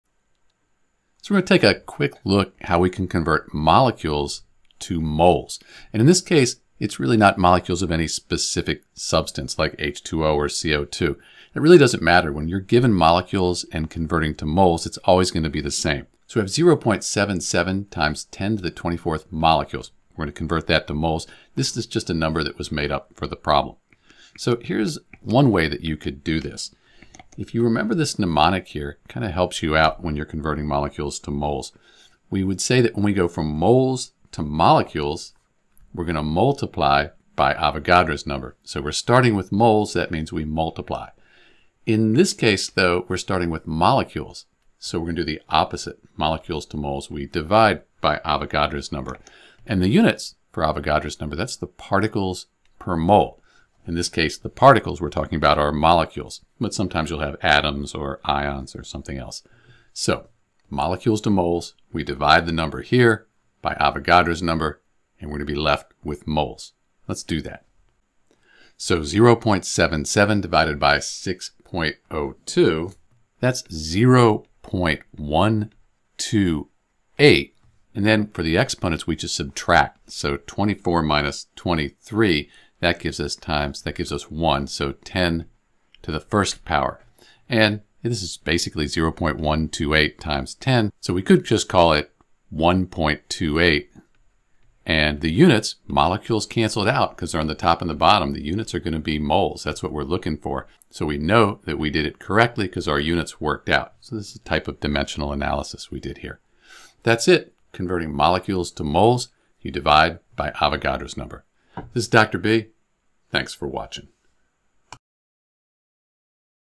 Convert Molecules to Moles
To convert 0.77 x 10^24 molecules (of any compound) to moles, we use Avogadro's number, which is approximately 6.022 x 10^23 molecules per mole.

Step 1: Determine the conversion factor
1 mole = 6.022 x 10^23 molecules

Step 2: Set up the conversion
0.77 x 10^24 molecules * (1 mole / 6.022 x 10^23 molecules)

Step 3: Calculate the result
0.77 x 10^24 / 6.022 x 10^23 = 1.28 moles

Step 4: Determine the correct significant figures
The original number, 0.77 x 10^24, has two significant figures, so the final answer should also have two significant figures. Therefore, the result is approximately 1.3 moles (rounded to two significant figures) of the compound.

For more practice and a better understanding of moles, grams, and other related concepts, you can check out the following resources:

For further resources and information related to chemistry, visit my chemistry